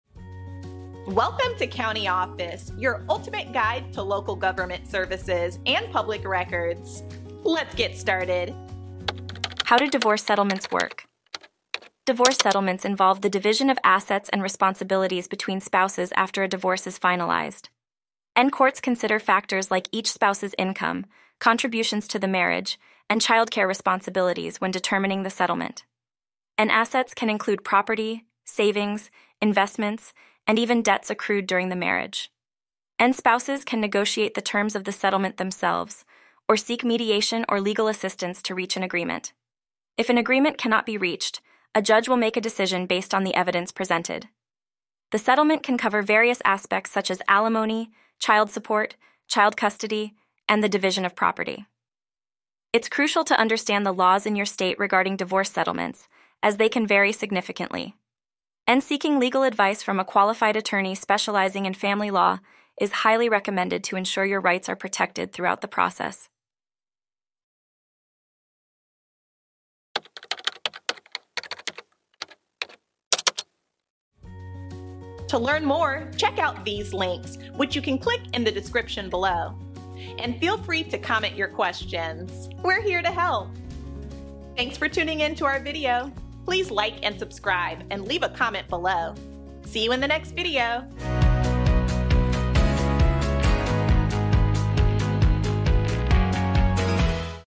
How Do Divorce Settlements Work? - CountyOffice.org
How Do Divorce Settlements Work? In this enlightening video, we delve into the intricate workings of divorce settlements, shedding light on the crucial aspects involved in the division of assets and responsibilities post-divorce. Courts meticulously analyze various factors like income, contributions, and childcare duties to reach a fair settlement. Assets ranging from property to debts accumulated during the marriage are subject to division. Whether through negotiation, mediation, or legal assistance, spouses strive to come to an agreement; otherwise, a judge steps in. Alimony, child support, custody, and property division are key components of these settlements. Understanding the nuances of divorce settlement laws specific to your state is paramount. Seeking guidance from a seasoned family law attorney is highly advisable to safeguard your rights during this process.